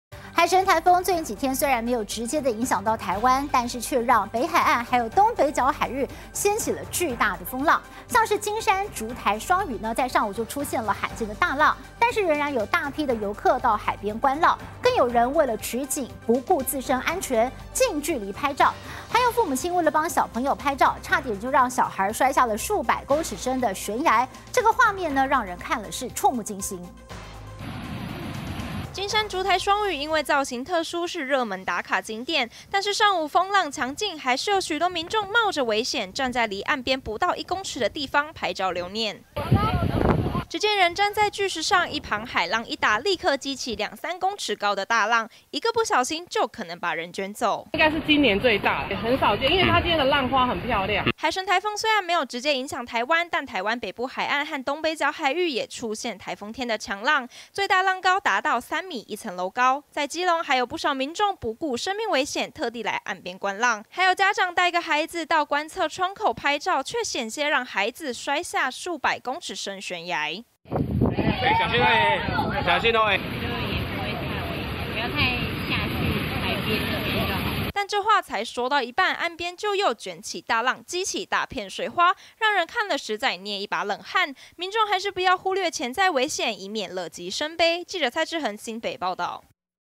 燭臺雙嶼罕見大浪 家長幫童拍照險墜崖
颱風海神，這幾天與台灣擦身而過，雖沒直接影響台灣，卻讓北海岸與東北角海域，掀起巨大風浪。金山燭台雙嶼，上午就出現罕見大浪，但仍有大批遊客到海邊觀浪，更有人為了取景，不顧自身安全，近距離拍照，還有父母為了幫小孩拍照，差點讓孩子摔下數百公尺深的懸崖，畫面讓人怵目驚心。